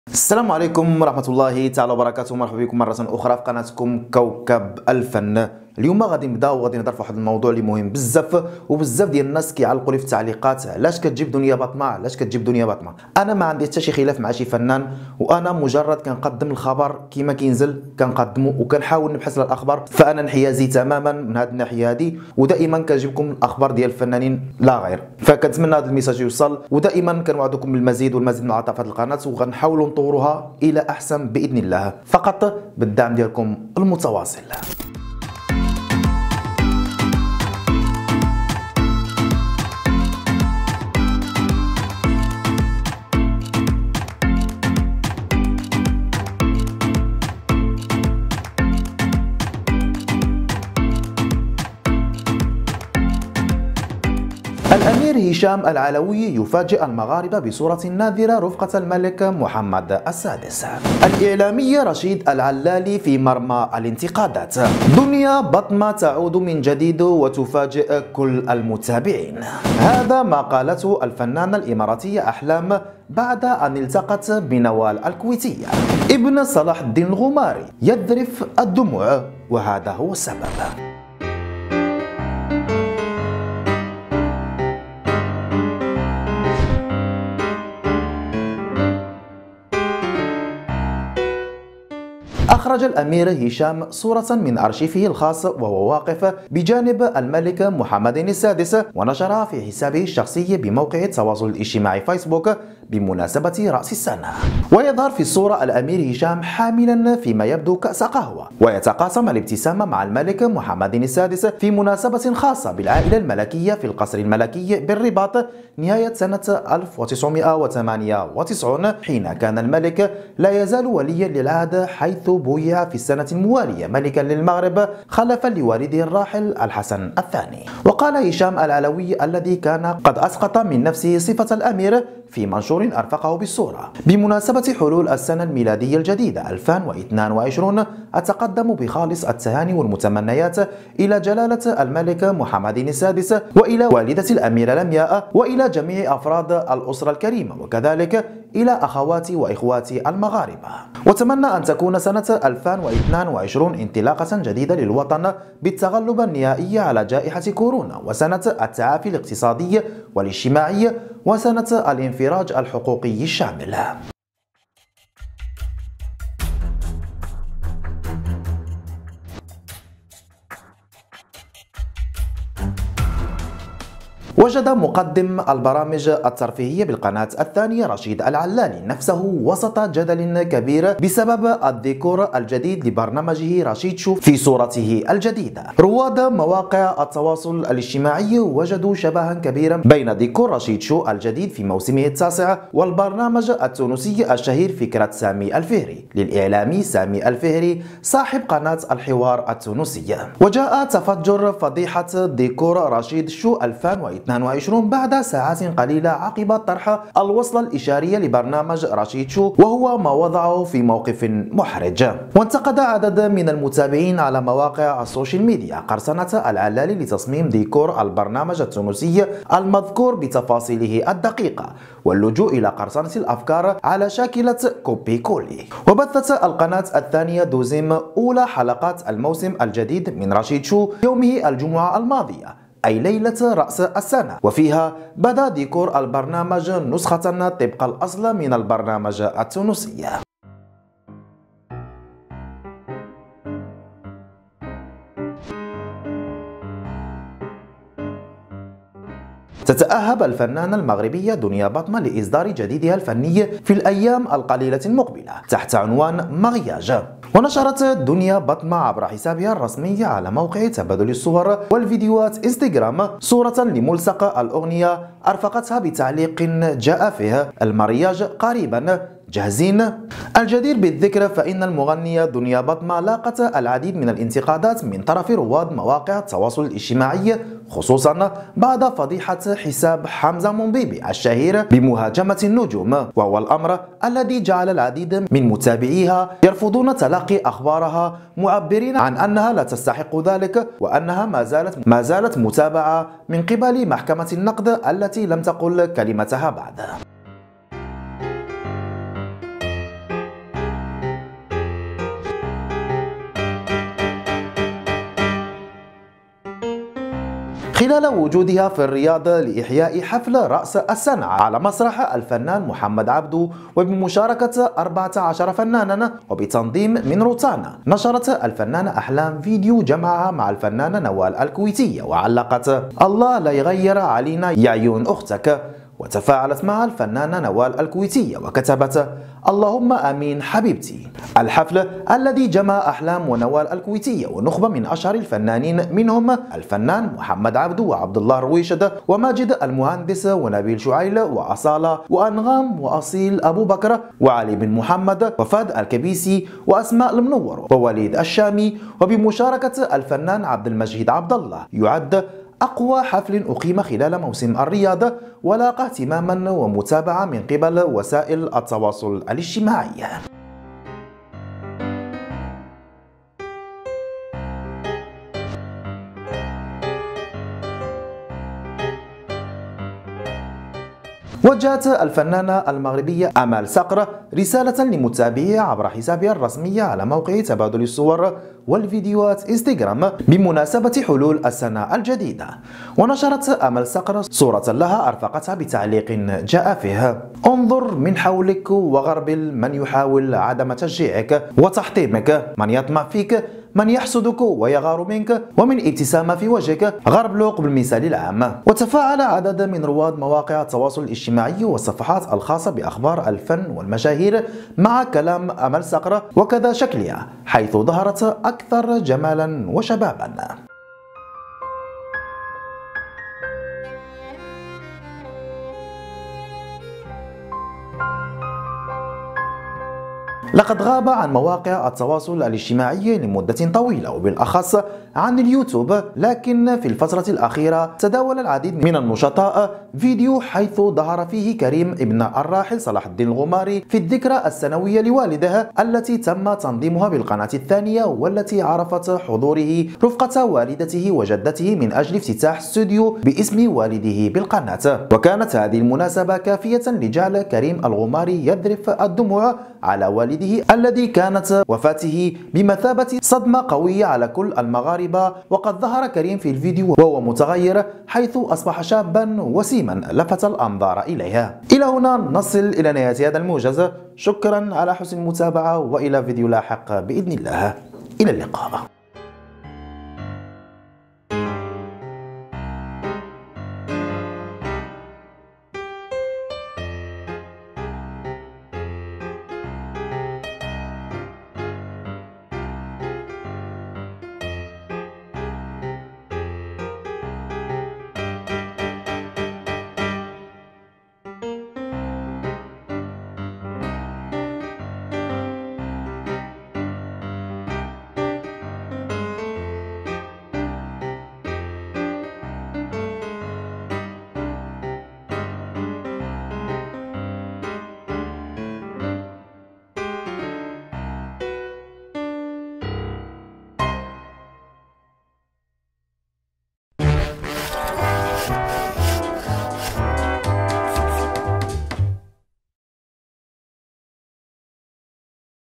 دنيا بطمة تفاجئهم/انتقادات رشيد العلالي/الملك محمد السادس يثير اعجاب المغاربة/أمل صقر وتدوينة غريبة
وسأكون شاكرا إذا أعجبك فيديو من فيديوهاتي
سبحان الله و بحمده سبحان الله العظيم ♥
# كوكب _الفن